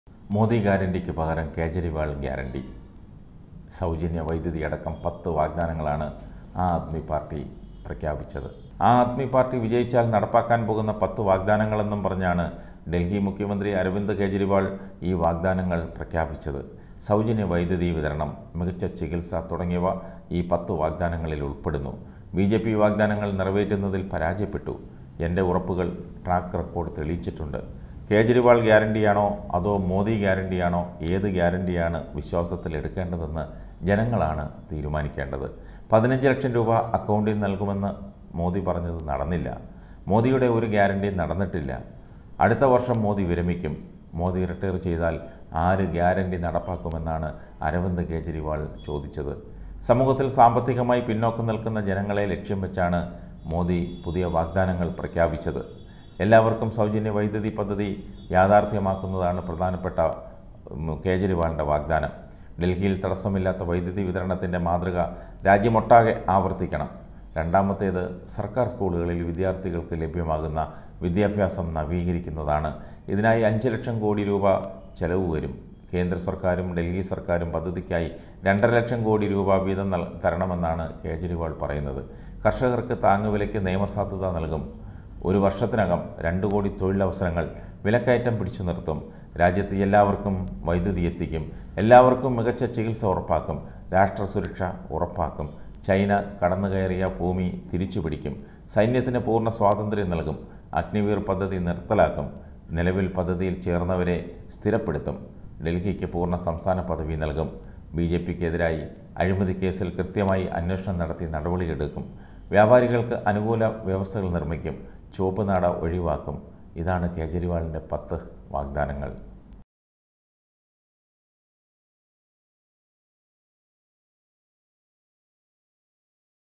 മോദി ഗ്യാരന്റി അറബികടലിൽ, പകരം കെജ്‌രിവാൾ ഗ്യാരന്റി|INDIA|CONGRESS|AAP|RAHULGANDHI|CPM|PINARAYI|BJP
എല്ലാവർക്കുംസൗജന്യ വൈദ്യുതിയുൾപ്പെടെ കെജ്‌രിവാളിന്റെ പത്ത് വാഗ്ദാനങ്ങൾ  | KEJRIVAL | | AAP | | BJP |